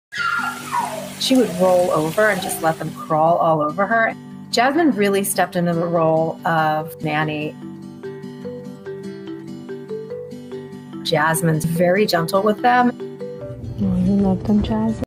100-Pound Dog Is The Perfect Nanny To Tiny Beagle Puppies |@TheDodo
@The Dodo  @Doggy Doggy Cartoons